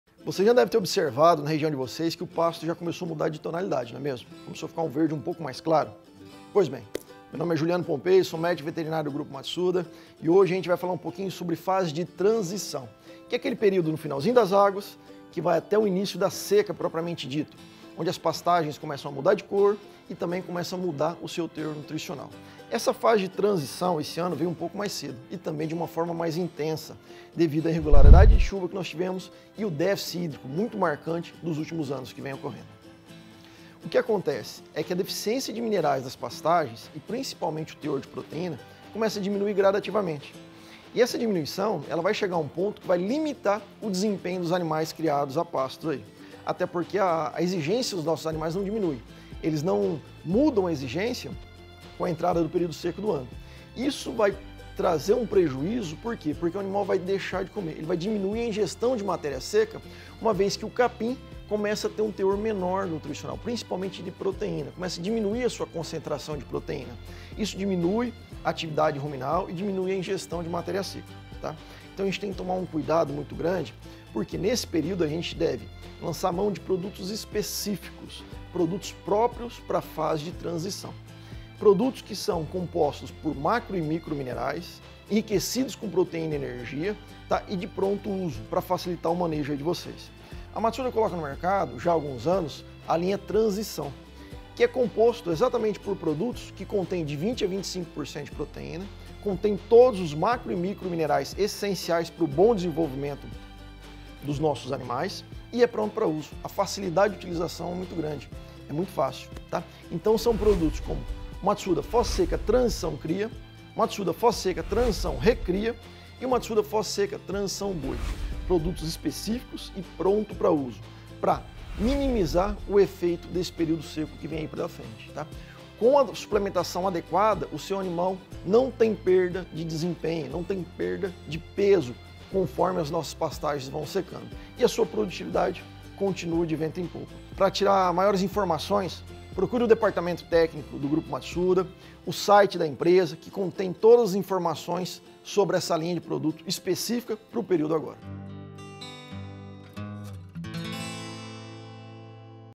Saiba o que fazer para que seus animais não percam peso no período de transição
O aproveitamento do período das águas, tal como a suplementação mineral correta no período de transição, podem garantir a rentabilidade da sua pecuária a pasto também no período seco!